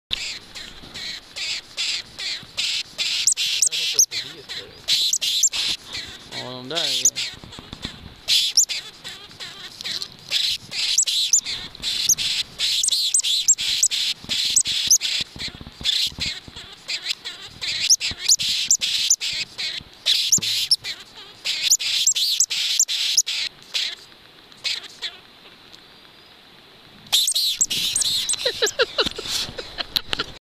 Very angry lemming attacking camera in Lappland Sweden.
In the end it jumps into the camera.